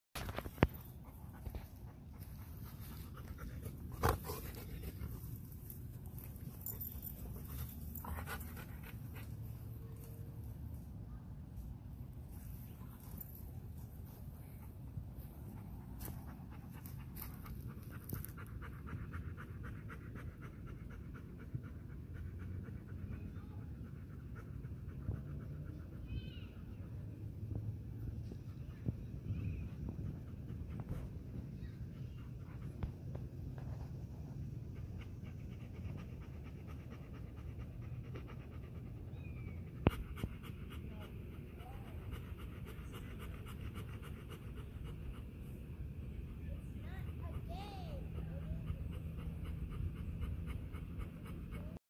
Golden Retriever Happy Rolling And Lying In The Grass
Jack happy rolling and lying in the grass.